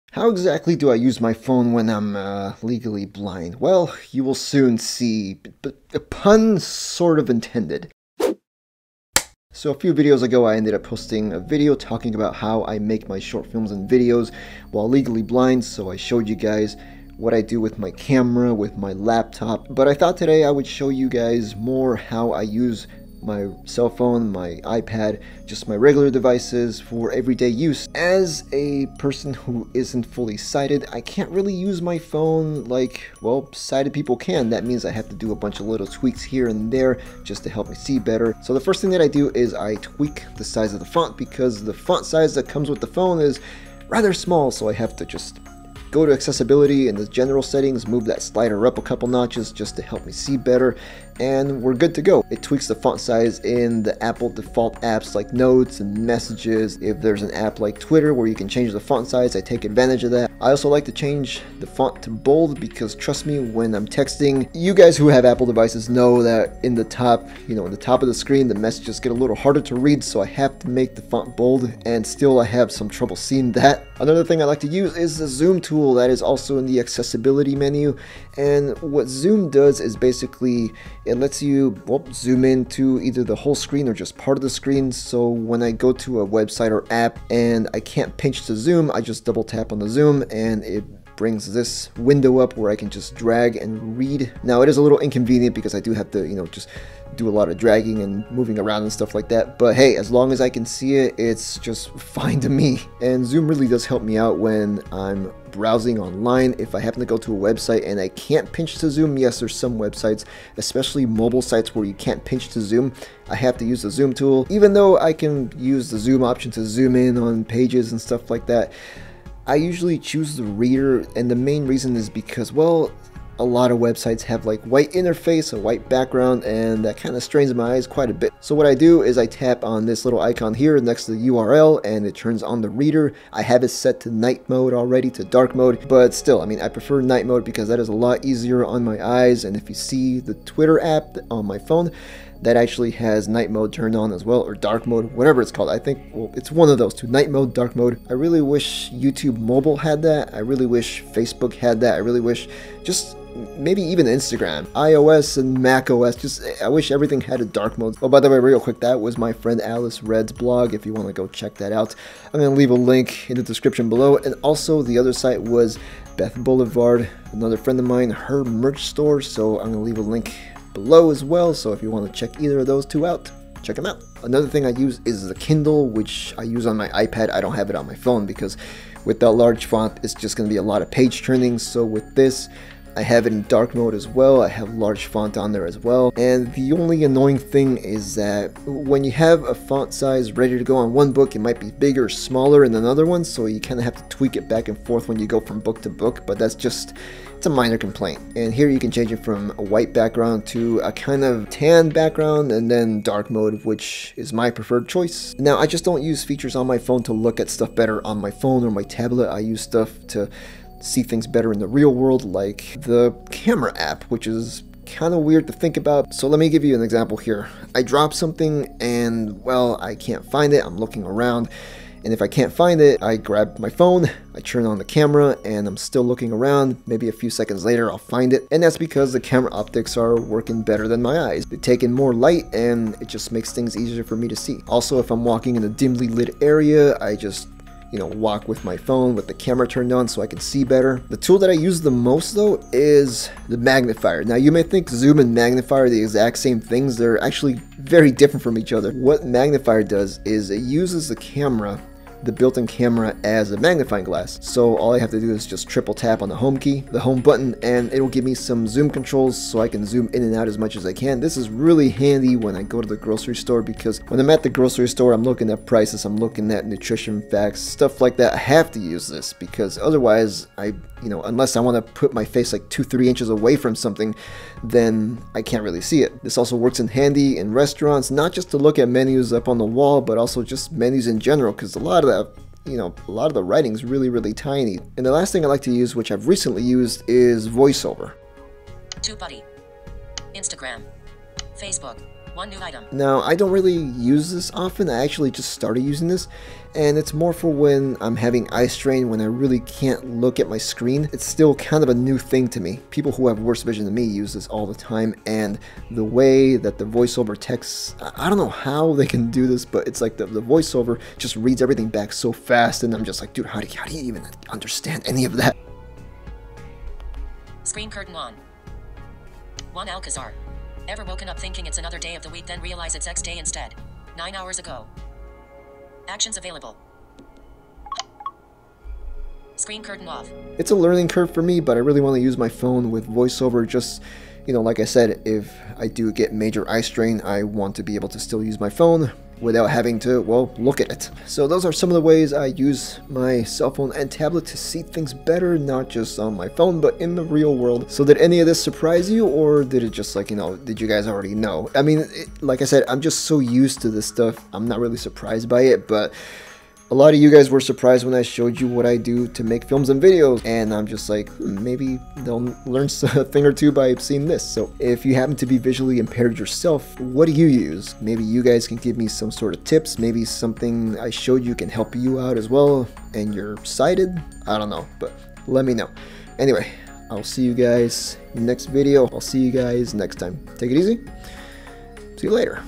How I use my phone as a legally blind person.
How does someone who's blind use a cell phone? Here I shows you the accessibility tools I use to help me use my iPhone and iPad better. There are several accessibility tools and shortcuts that a visually impaired cell phone user can use. I can only really speak for myself here since blindness and visual impairment covers such a wide spectrum, so some tools will benefit others more.

Links to the webpages featured below.